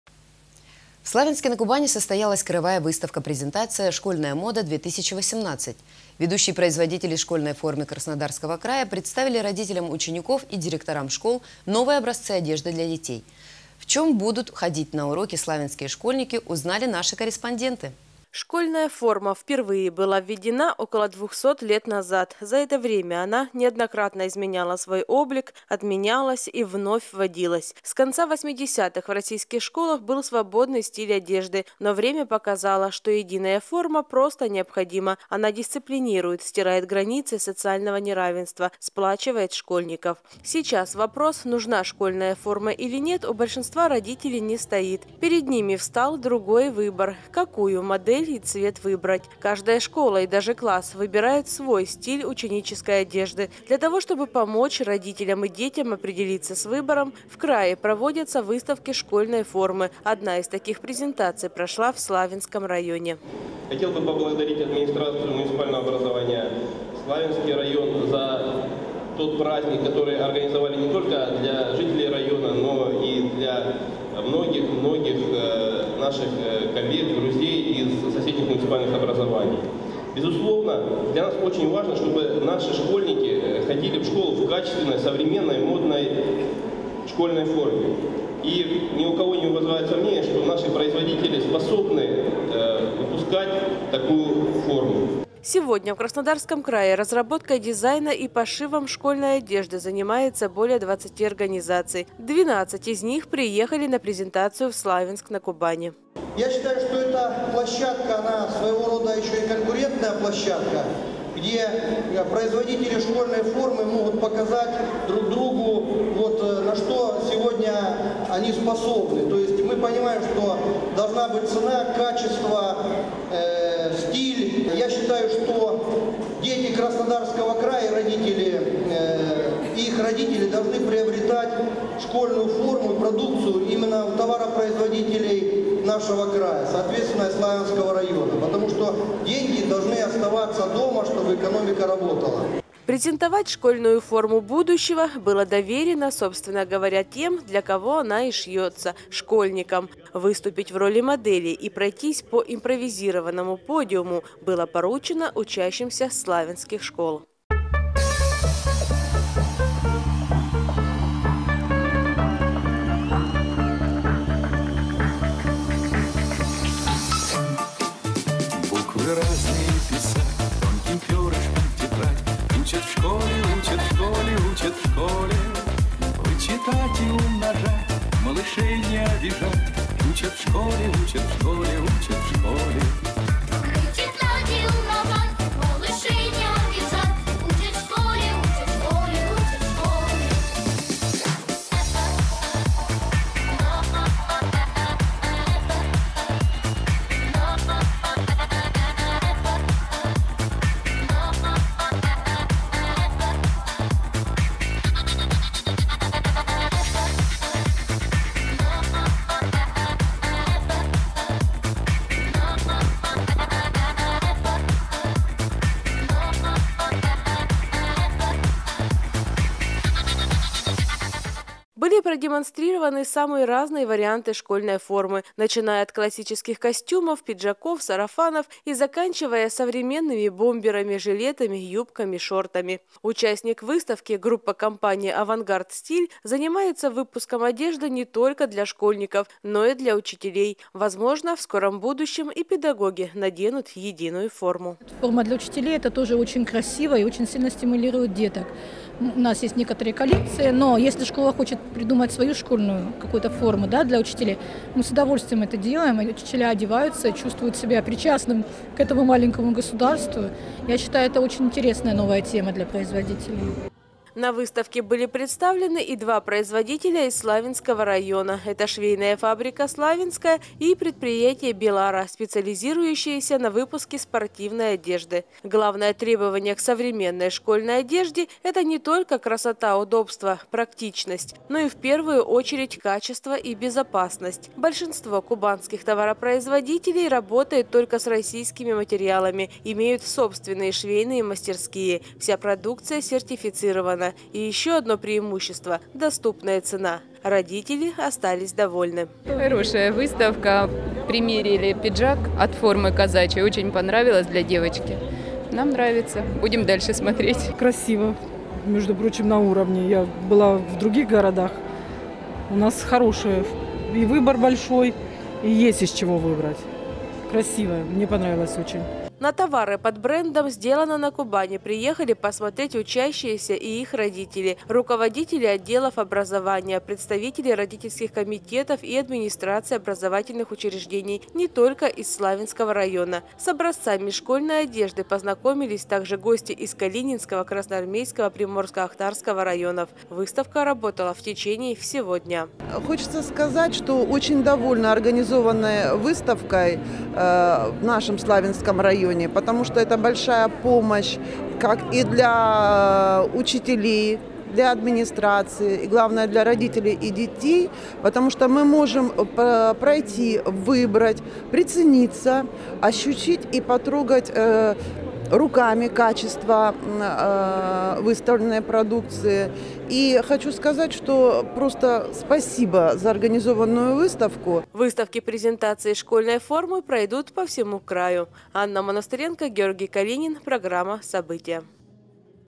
В Славянском районе прошла краевая выставка презентация школьной формы
В Славянске-на-Кубани состоялась краевая выставка-презентация «Школьная мода-2018». Ведущие производители школьной формы Краснодарского края представили родителям учеников и директорам школ новые образцы одежды для детей. В чем теперь будут ходить на уроки славянские школьники, узнали наши корреспонденты.